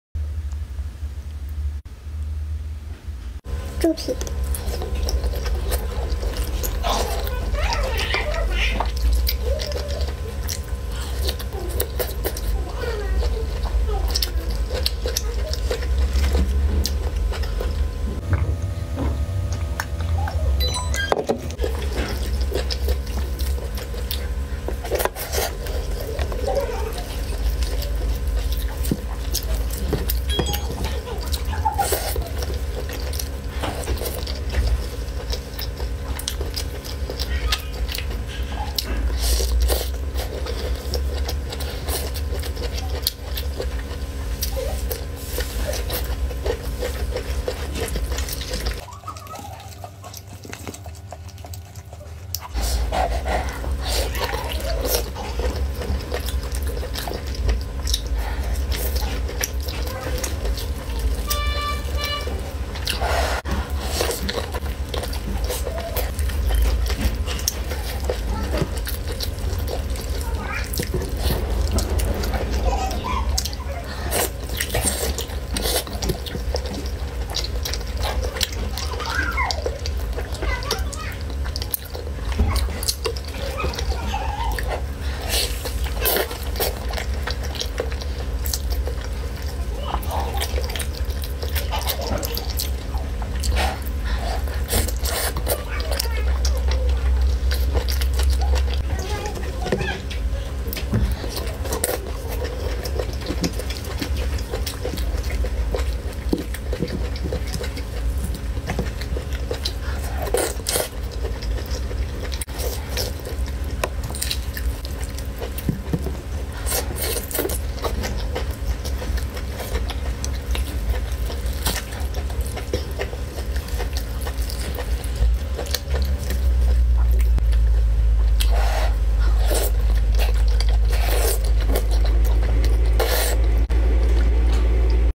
ASMR SEAFOOD BOIL 미국식 매운 해물찜 먹방 *랍스터, 새우, 꽃게, 전복 LOBSTER TAIL, SHRIMP, CRAB EATING SOUNDS MUKBANG
Hi. Today I cooked spicy seafood by adding Indonesian sambal sauce.
(Argentine shrimp, lobster tail and Indonesian sambal sauce.)

Translated using Google Translator
Therefore, if the translation is wrong, I would appreciate your understanding.